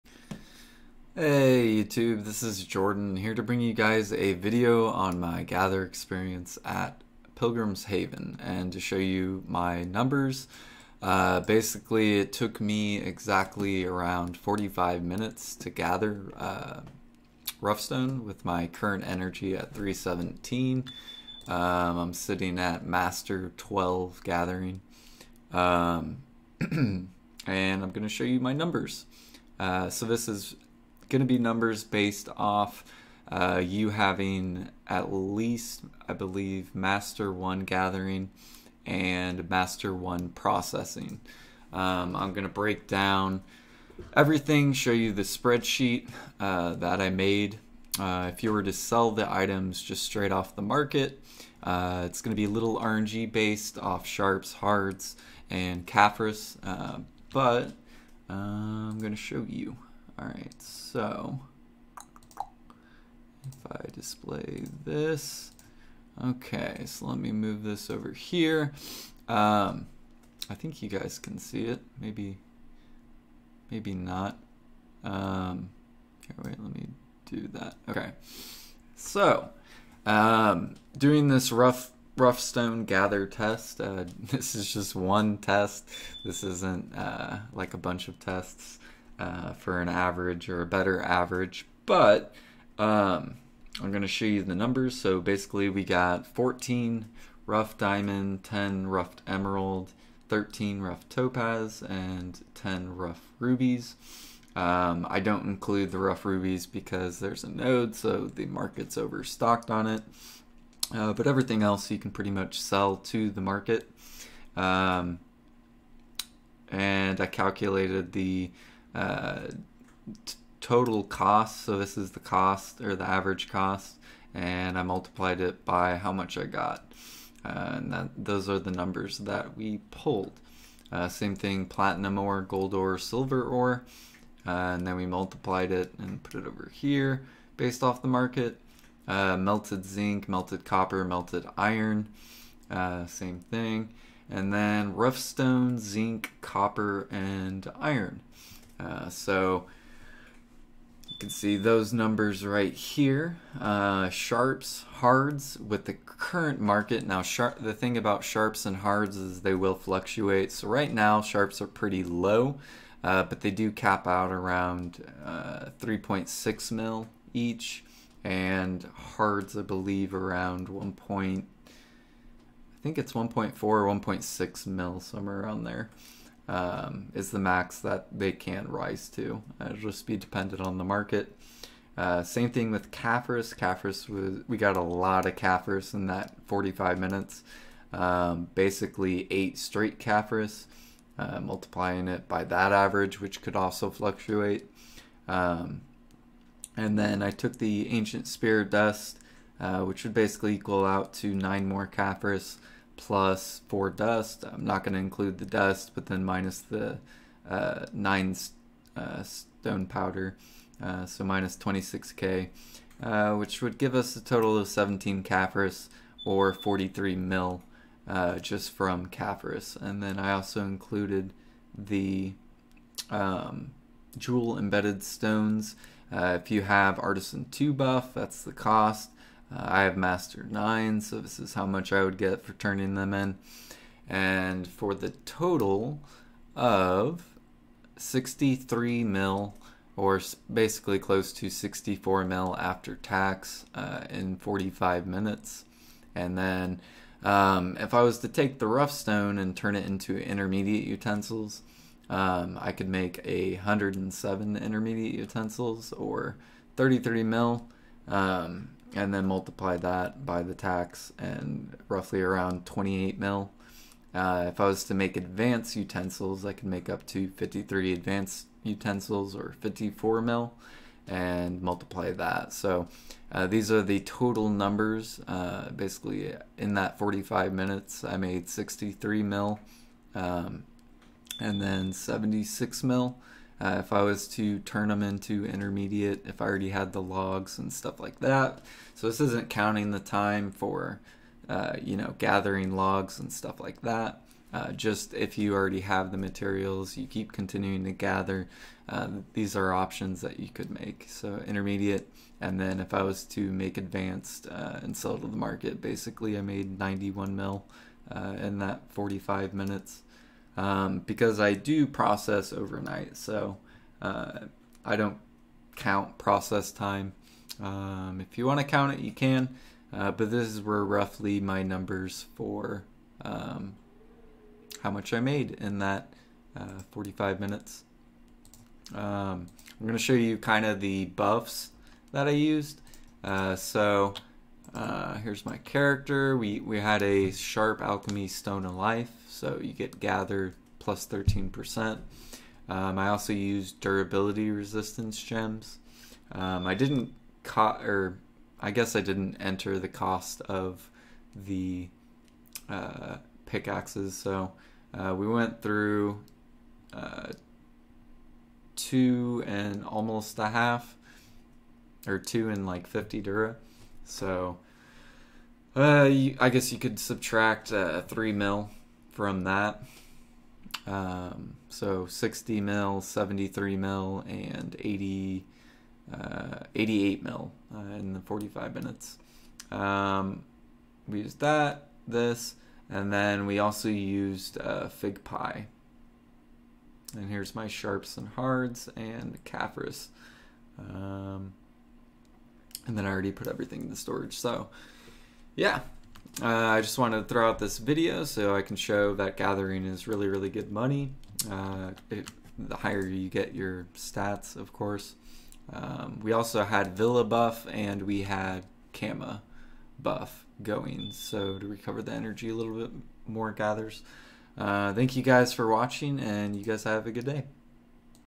60mil+ 45min Gathering Rough Stone Test Pilgrims Haven (T4 Hedge) non weekend bonus.
Requires Master 1 + Gathering Master 1 + Processing.

T4 Hedge Hog.
Lucky Tools Used in video
Sharp alchemy stone of life = 13% gathering
Fig pie = 3% Gathering
Villa
Kama Buff